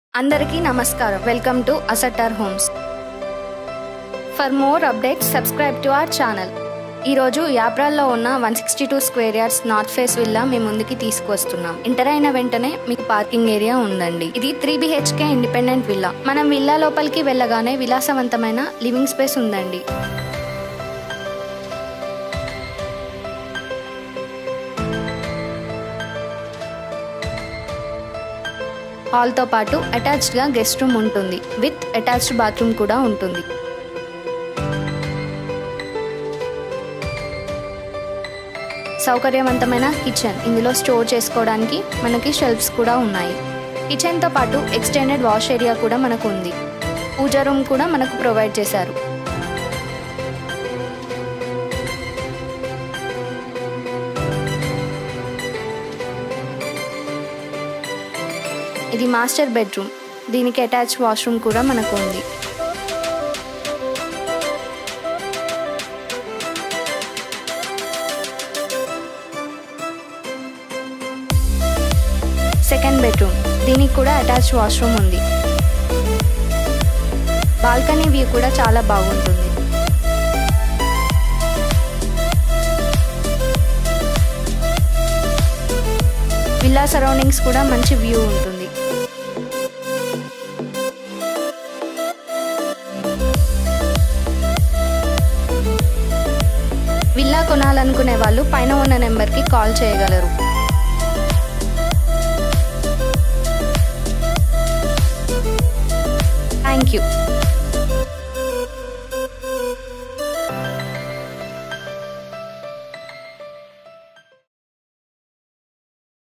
New Luxurious 3BHK Duplex Villa for Sale | Acertar Homes | House for sale in Hyderabad
You are looking for new 3bhk villa in Yapral, Secunderabad, please call the above number.
* 162 sq. yards
* North Face
* 3BHK
*1.18cr(negotiable)
* Near to Sainikpuri
* Full Secured
* Family Residency all around